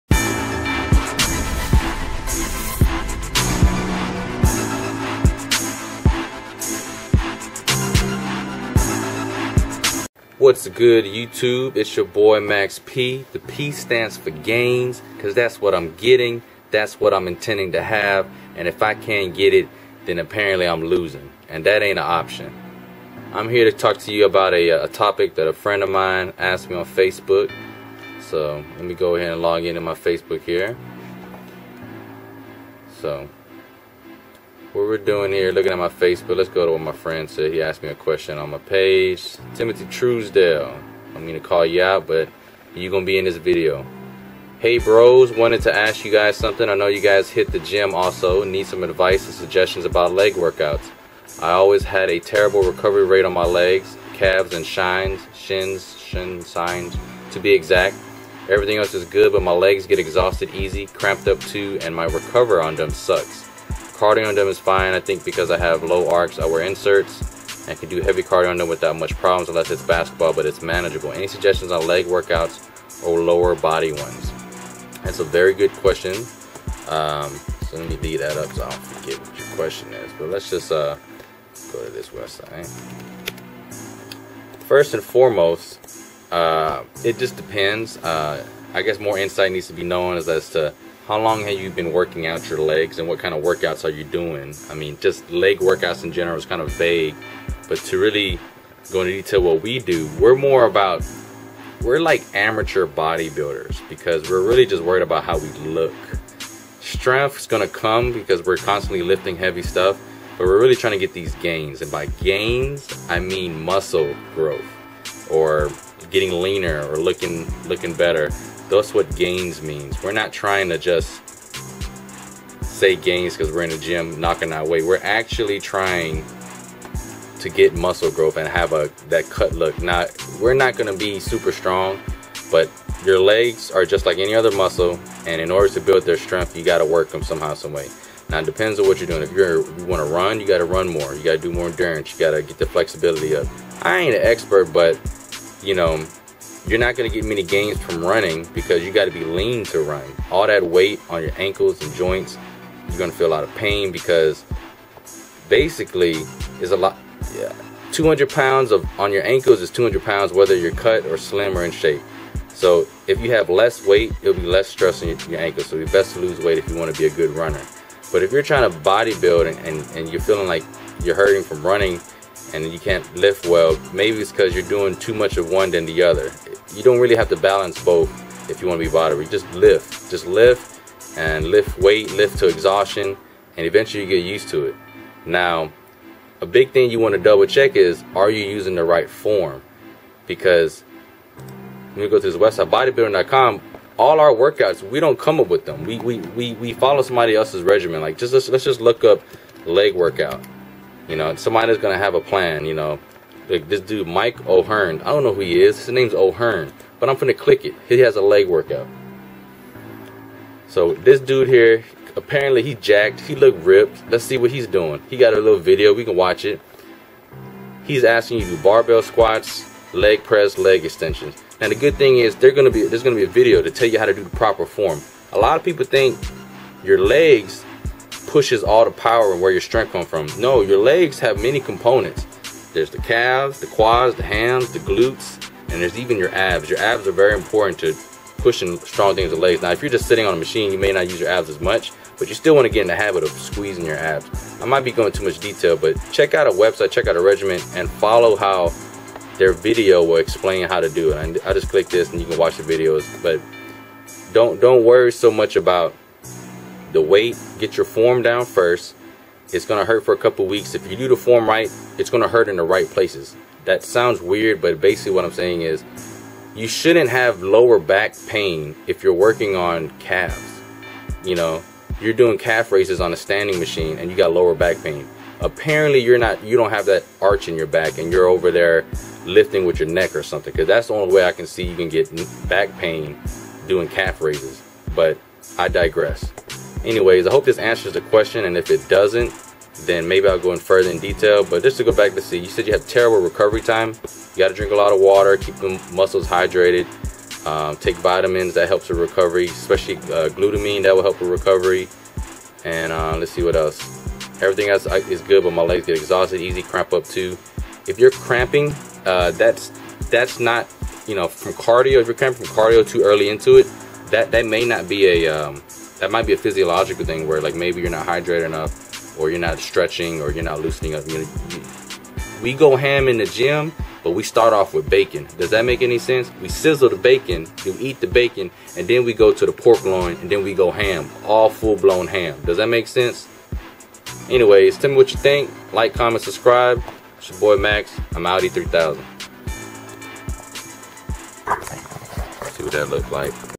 What's causing leg fatigue? RushHour Fit
Friend asked what can he do to recover after his leg workouts.
Drink more fluids to prevent cramping throughout the day.

Follow a regiment and be sure to work on form prior to adding weight.